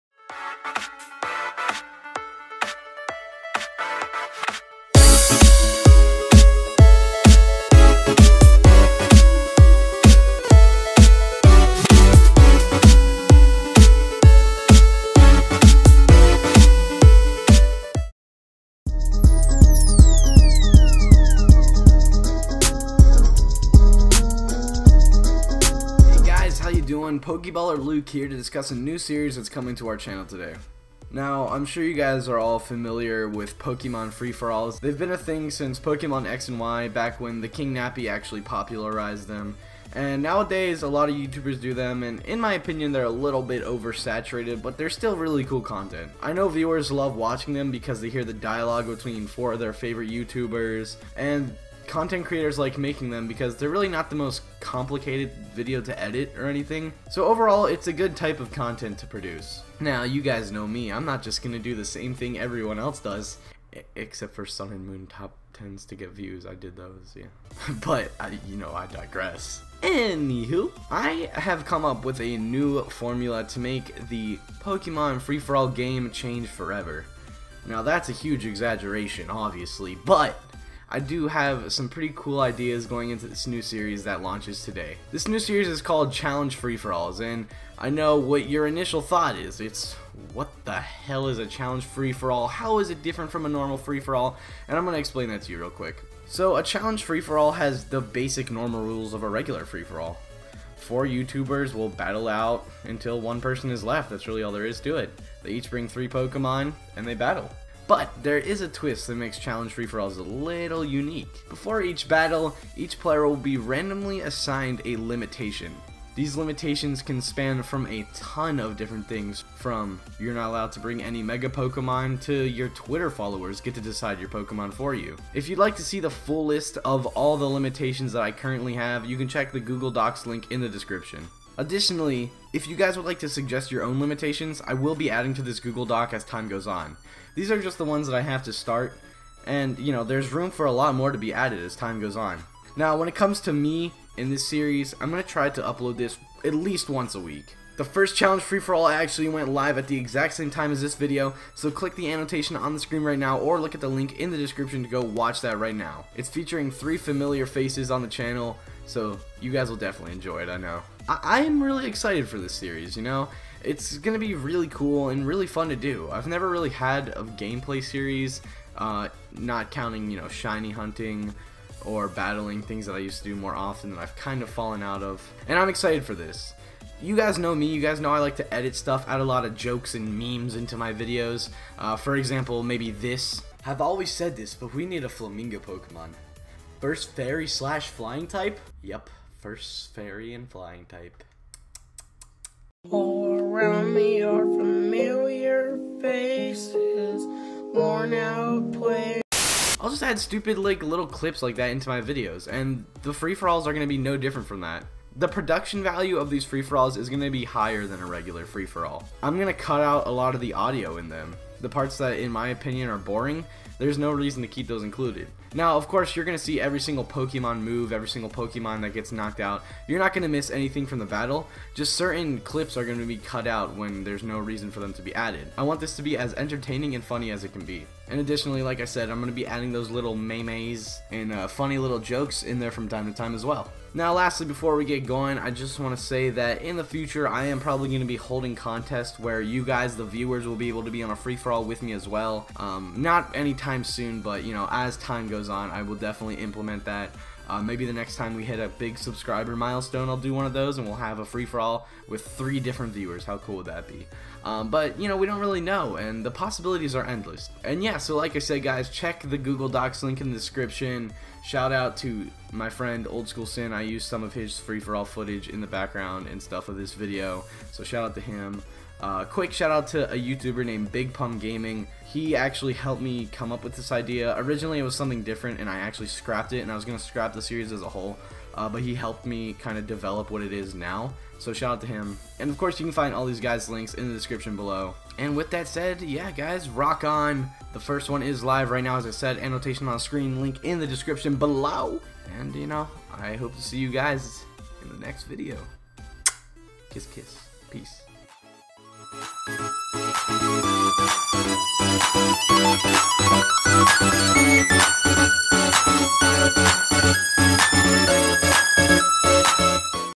Introduction to Challenge Free For All - FFAs Will Never Be The Same
Thanks for all the support recently! I have a strong hope in this series! It's gonna be very fun! I can't wait to invite tons of guests onto my channel to battle! Leave challenge suggestions in the comments below!